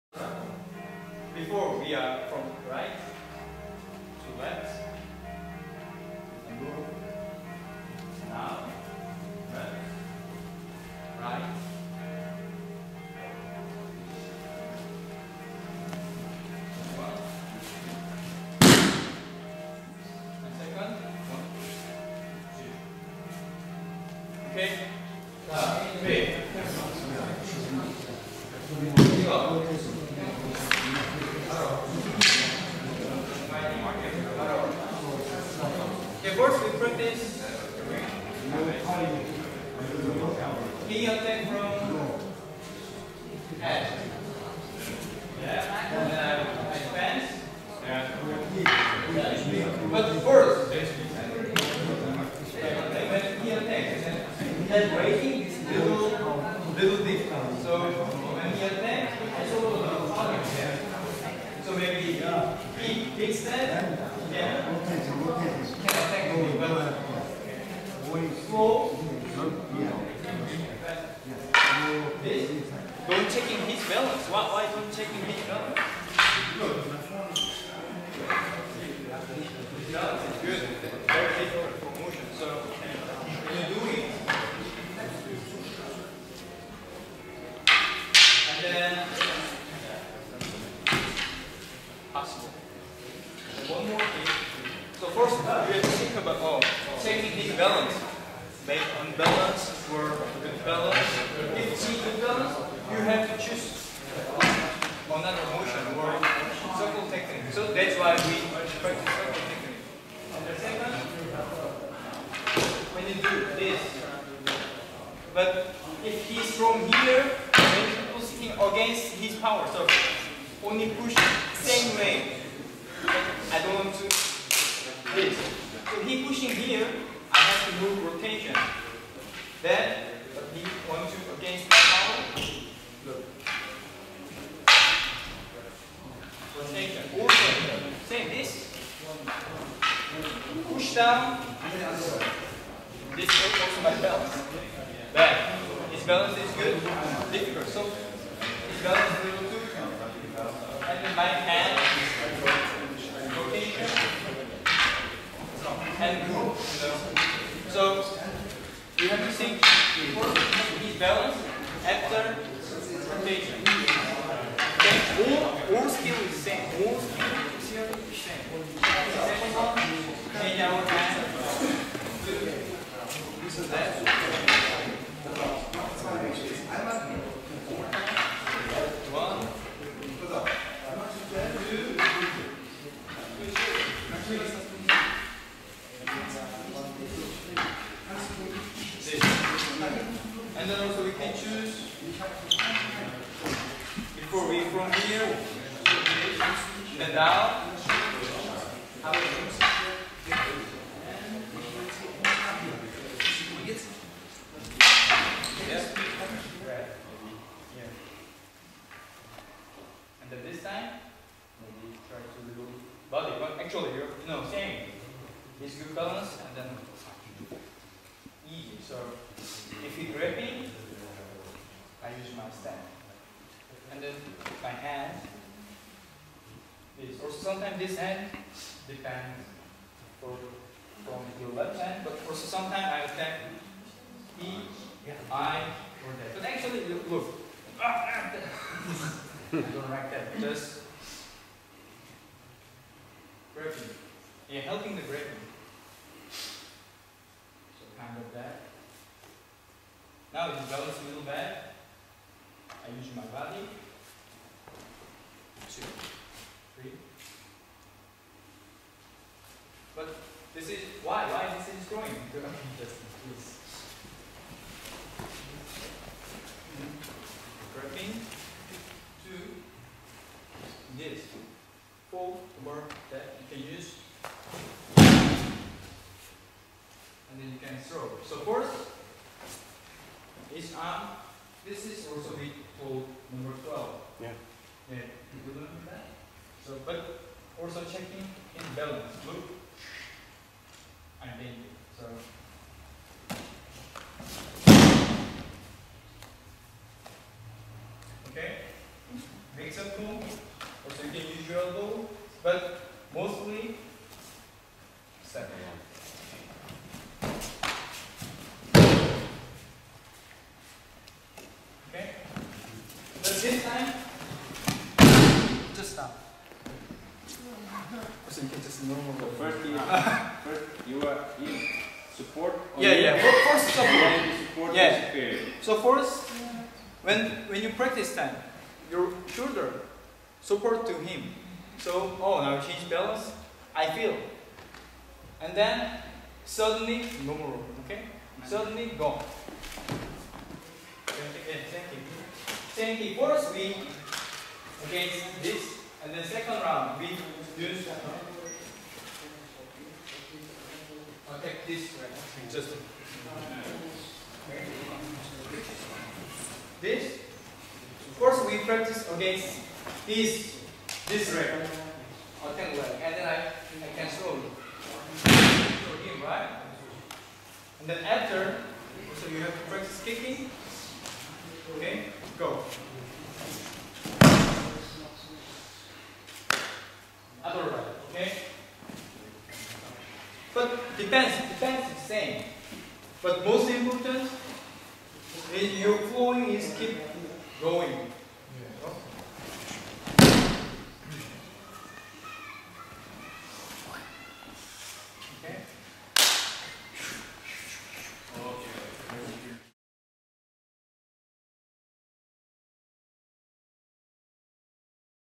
European Hankido Seminars 2011
Hankido techniques taught during the European Hankido Seminars in 2011.
Instructor Ko Se-hwan of Sangmukwan in Seoul teaches and demonstrates several hankido techniques.